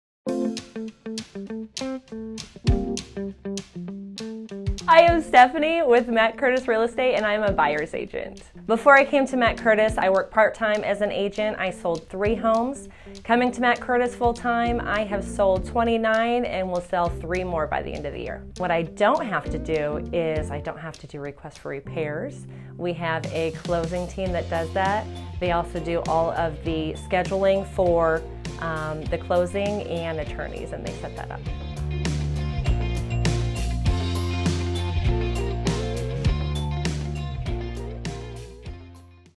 Why I Chose Matt Curtis Real Estate | Stephanie Kritzer's Story
Call Matt Curtis Real Estate today at 256.333.MOVE!

Visit Matt Curtis Real Estate in person:
8097 Madison Blvd, Suite 103
Madison, Alabama 35758